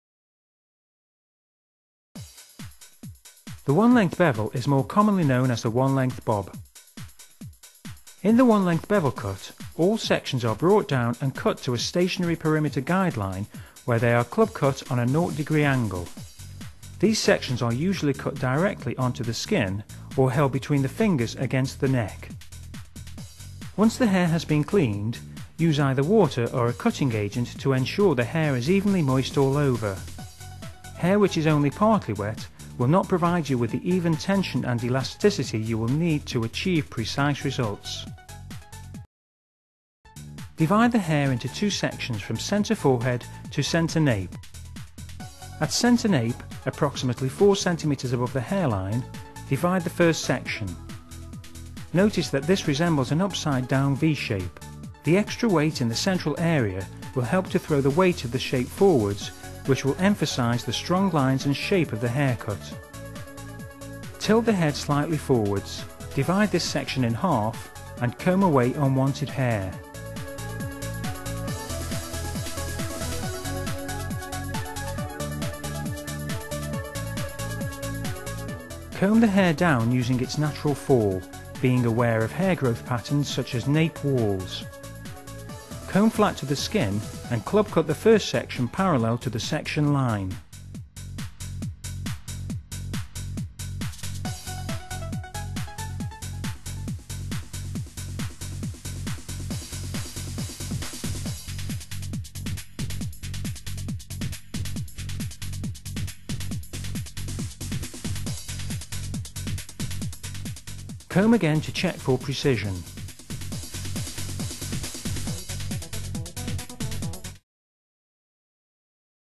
Hair Dressing 'How To' Video 4 of 6 : One Length Bevel (Bob)
Short teaser video of how to perform a one length bevel (Bob) cut.

Hairdresser: Greg Woolliscroft
Narrator: Nick Beech